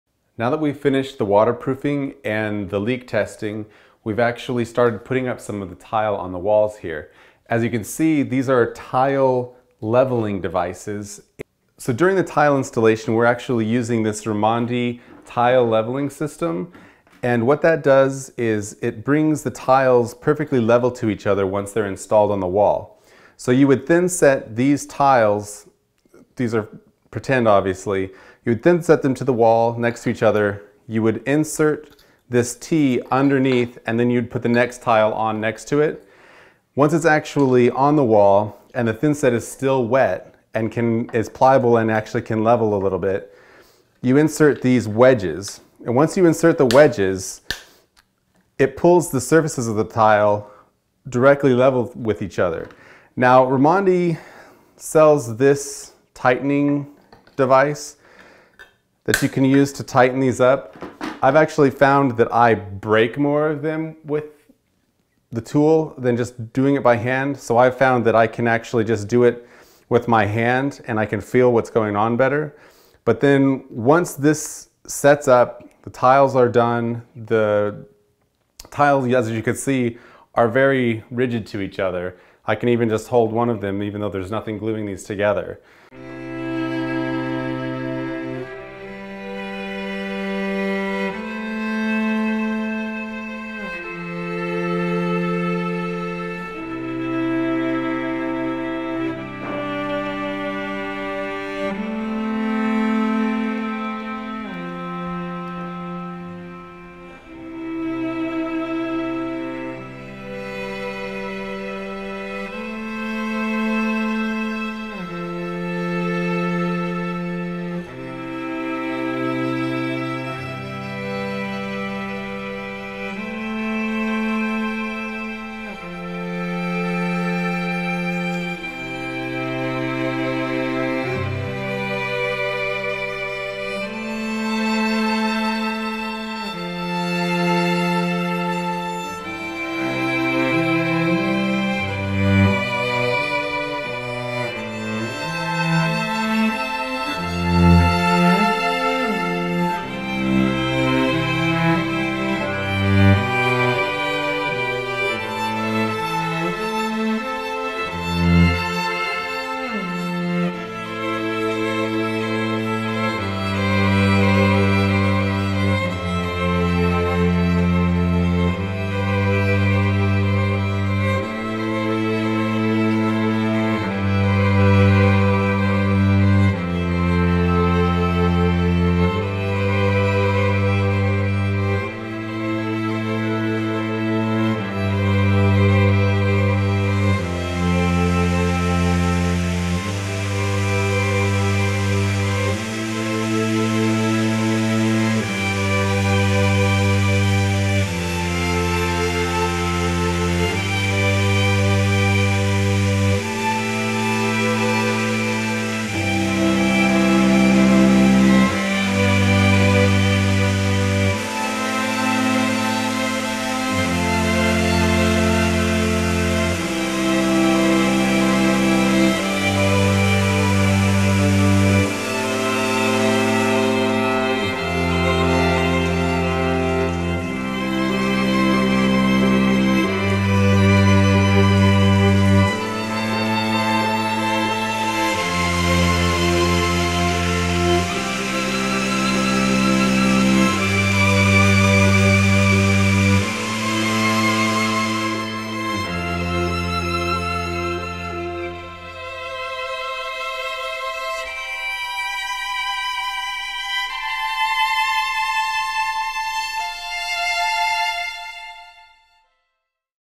Making a tiled bathtub AKA a roman tub, Episode 3/3
In this episode we start our bathroom project off by creating a Roman tub. Think indoor hot tub with tile. The concept is you create a very large water tight tile area that can be filled with water. It allows you to make a tub that can be nearly any size or shape you can imagine.